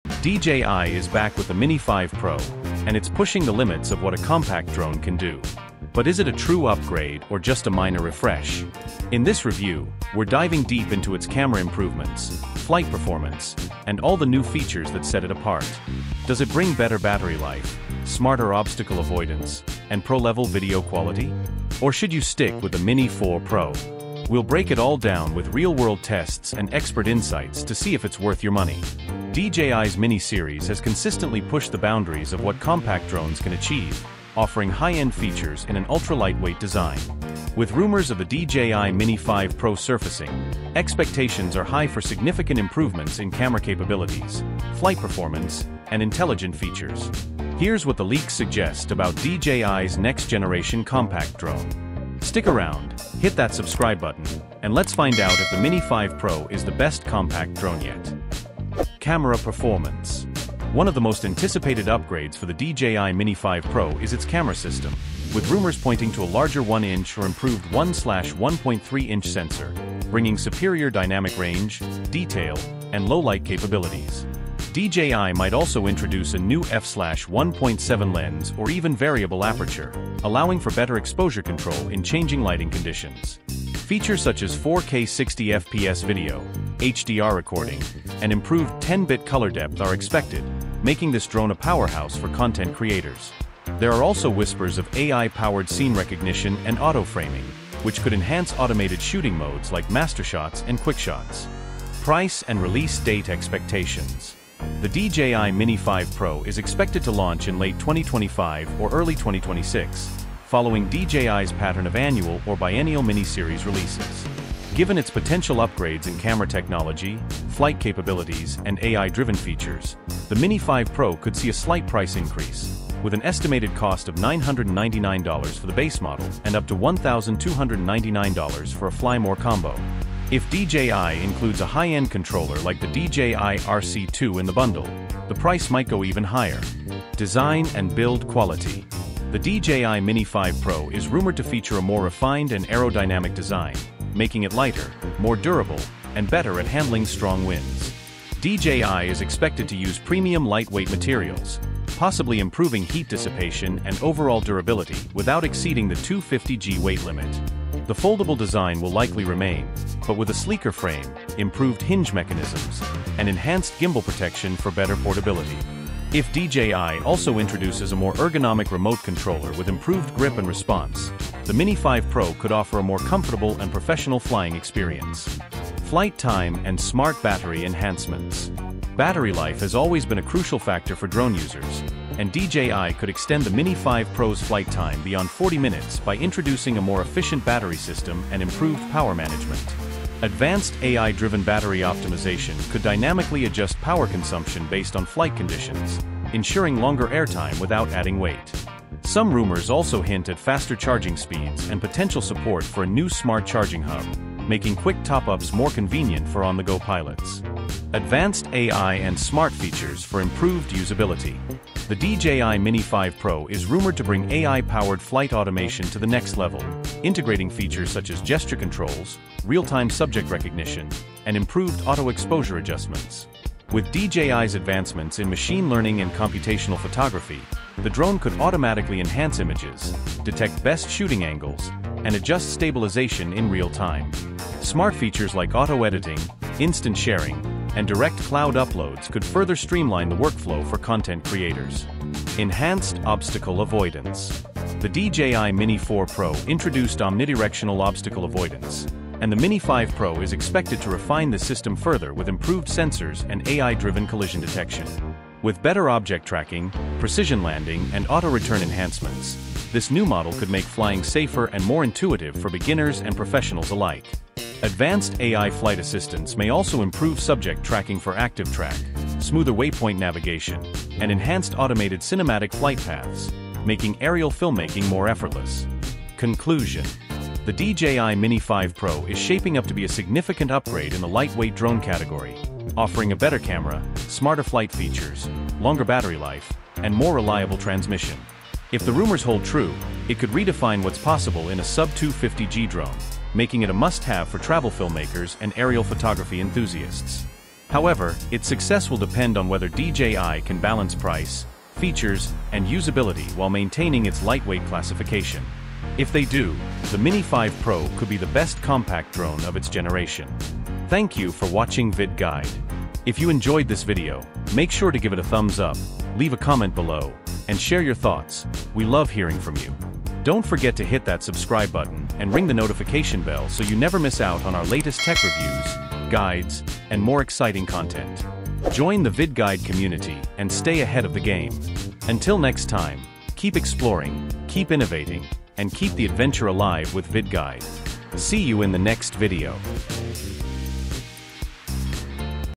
DJI Mini 5 Pro - Specs, Price, & Release Date Revealed!
Welcome to VidGuide! The DJI Mini 5 Pro is rumored to be DJI’s next-generation ultra-light drone, packed with enhanced camera capabilities, improved flight time, and smarter AI-powered obstacle avoidance. Could this be the best sub-250g drone for creators and travelers?

We’ll explore the expected specs, camera upgrades, battery life, pricing rumors, and release date leaks to see if the Mini 5 Pro is worth the upgrade. Will it set a new benchmark for compact drones? Watch now to find out!